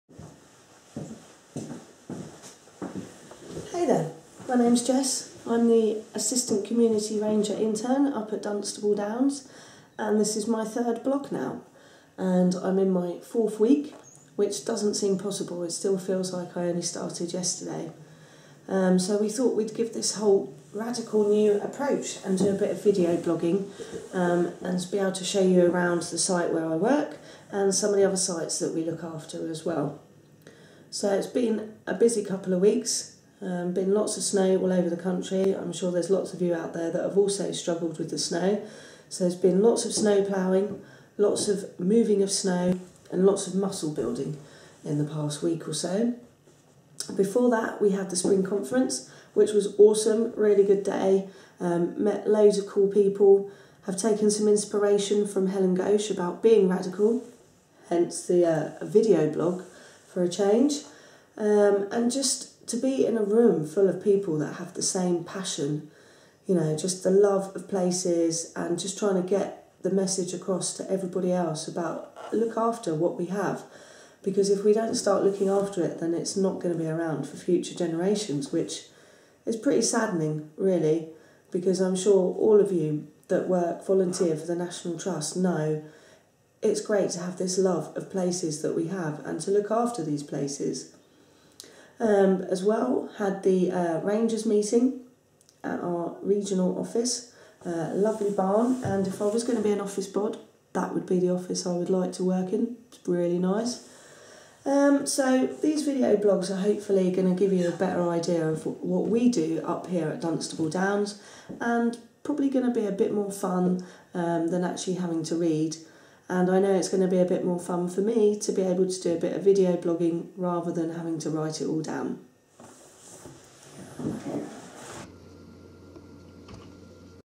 Introducing Jess
A video blog from Jess. The Community Ranger Intern for Bedfordshire